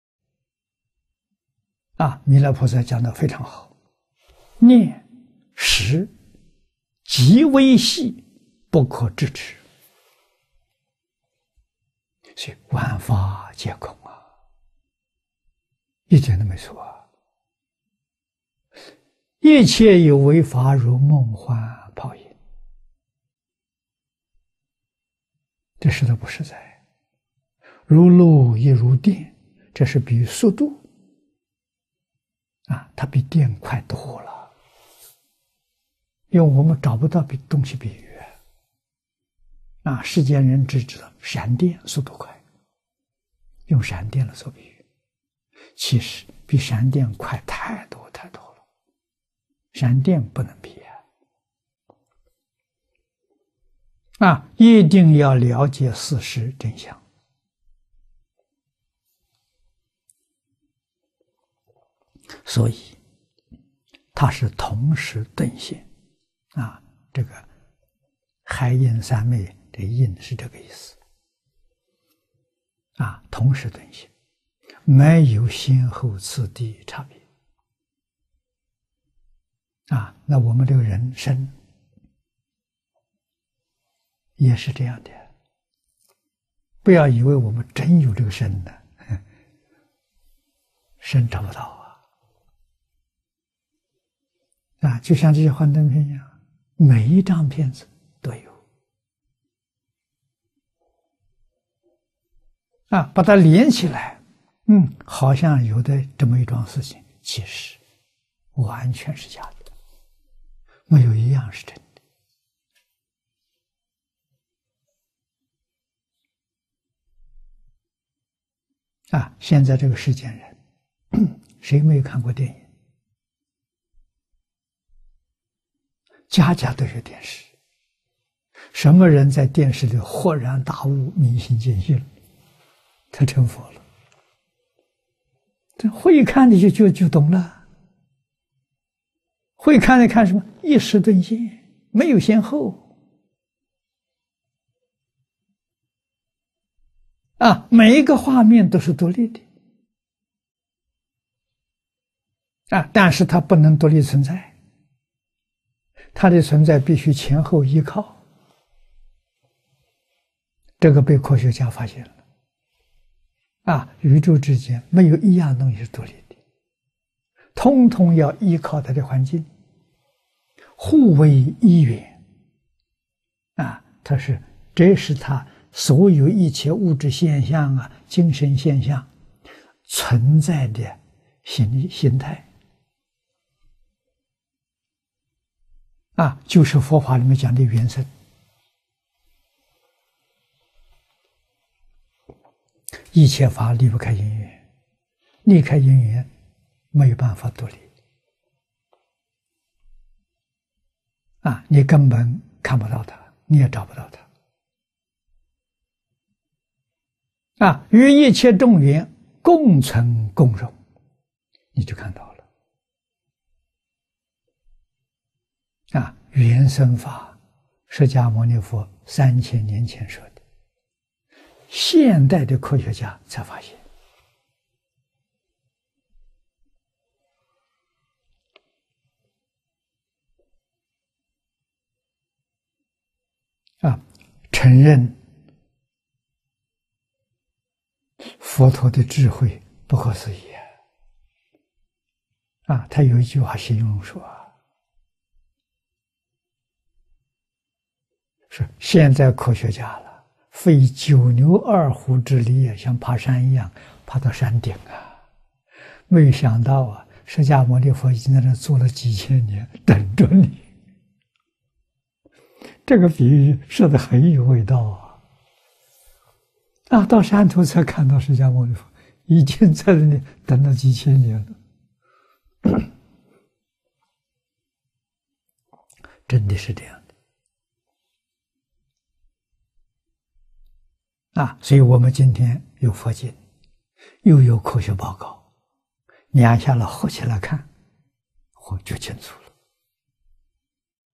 現代科學家發現緣生法
彌勒菩薩講得非常好，「念識極微細，不可執持」，所以萬法皆空，一點都沒錯。「一切有為法，如夢幻泡影」，這說它不實在，「如露亦如電」，這是比喻速度。它比電快多了，因為我們找不到東西比喻，世間人只知閃電速度快，用閃電來做比喻。其實比閃電快太多太多了，閃電不能比，一定要了解事實真相。所以它是同時頓現，海印三昧這個印是這個意思，同時頓現，沒有先後次第差別。我們這個人身也是這樣的，不要以為我們真有這個身，身找不到。就像這些幻燈片一樣，每一張片子都有，把它連起來好像有這麼一樁事情，其實完全是假的，沒有一樣是真的。現在這個世間人誰沒有看過電影？家家都有電視，什麼人在電視裡豁然大悟、明心見性，他成佛了。會看的就懂了，會看的看什麼？一時頓現，沒有先後。每個畫面都是獨立的，但是它不能獨立存在，它的存在必須前後依靠。這個被科學家發現了，宇宙之間沒有一樣東西是獨立的，統統要依靠它的環境，互為依緣。他說這是所有一切物質現象、精神現象存在的形態，就是佛法裡面講的緣生。一切法離不開因緣，離開因緣沒有辦法獨立，你根本看不到它，你也找不到它。與一切眾緣共存共榮，你就看到了。
　　緣生法，釋迦牟尼佛三千年前說的，現代的科學家才發現，承認佛陀的智慧不可思議。他有句話形容說，說現在科學家費九牛二虎之力，像爬山一樣爬到山頂，沒有想到釋迦牟尼佛已經在那裡坐了幾千年，等著你。這個比喻說得很有味道，到山頭才看到，釋迦牟尼佛已經在那裡等了幾千年，真的是這樣的。所以我們今天有佛經，又有科學報告，兩項合起來看，就清楚了。

文摘恭錄—淨土大經科註　（第九十三集）　2011/12/21　 檔名：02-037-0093